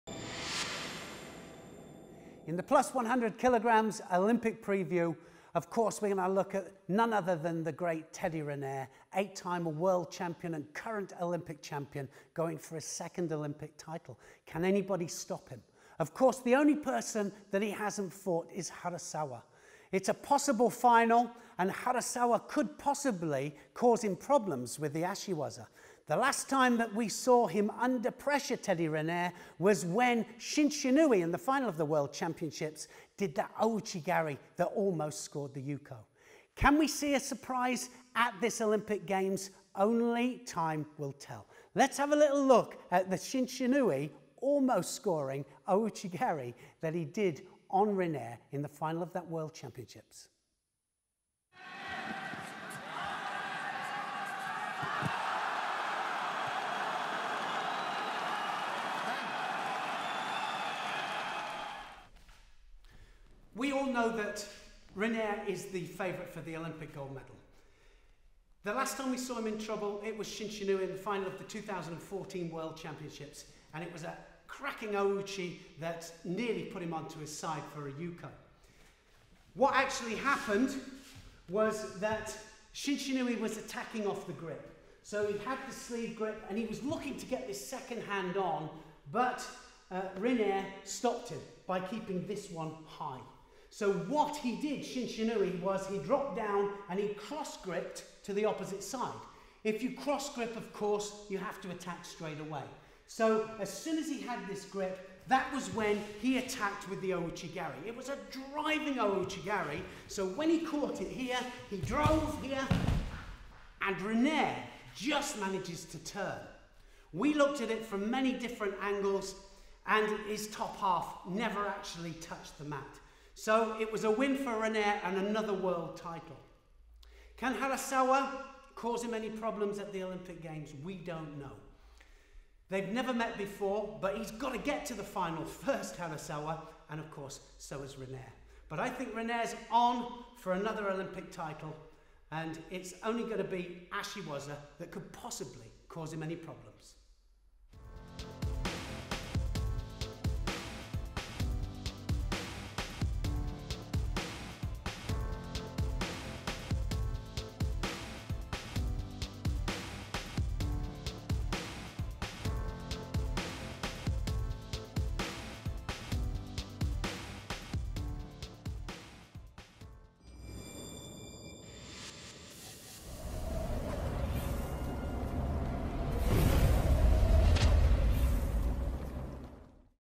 +100kg Category Preview - RINER
+100kg Category Preview with Neil Adams​. Focus is on defending champion and 8 times World Champion Teddy Riner of France!